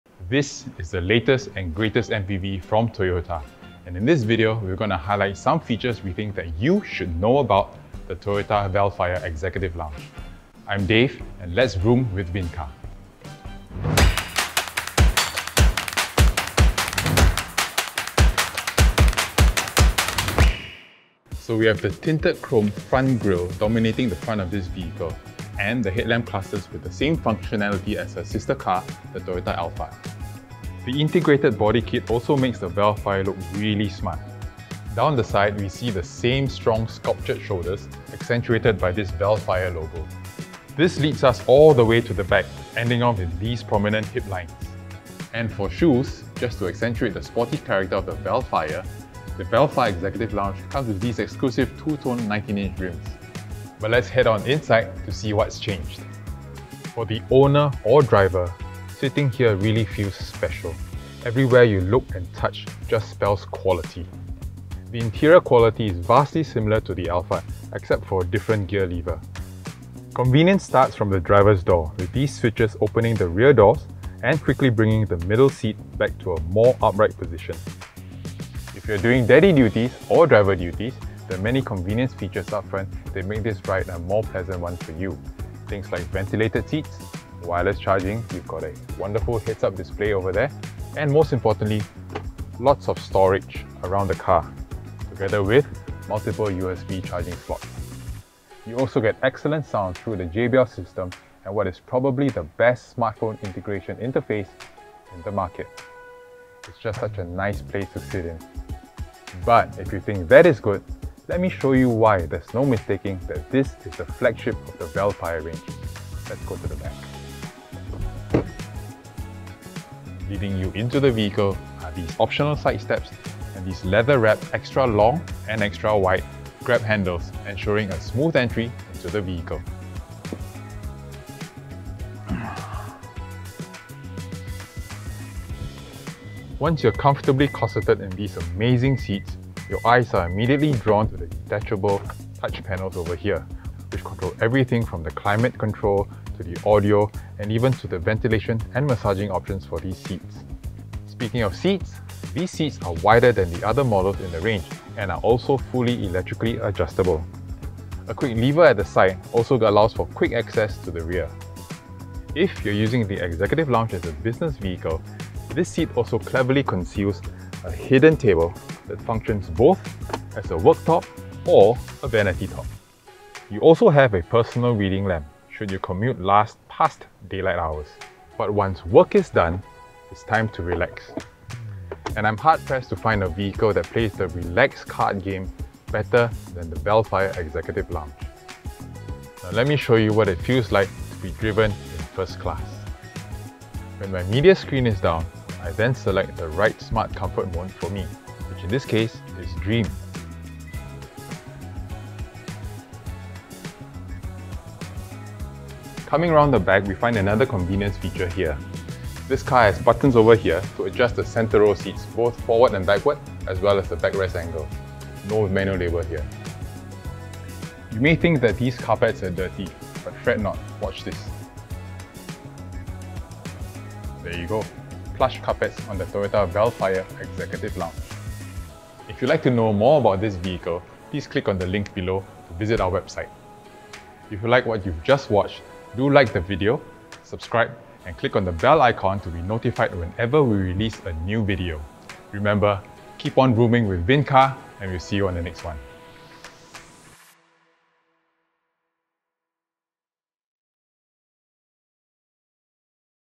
2024 Toyota Vellfire Executive Lounge | VROOM SPECIALS
The Toyota Vellfire Hybrid represents the essence of tailor-made luxury travel, serving as your reliable companion during your journey. Enter a world of unrivaled comfort and grandeur, meticulously crafted to awaken your senses.

With advanced safety enhancements enveloping you and your loved ones in a protective embrace, and propelled by an exceptional self-recharging hybrid engine, each journey evolves into a refined indulgence, transforming your travels into genuinely sumptuous experiences.

Chapters:
0:00 Introduction
0:21 Exterior
0:57 Front Interior
1:59 Middle Interior
3:26 Rear Interior
3:49 Conclusion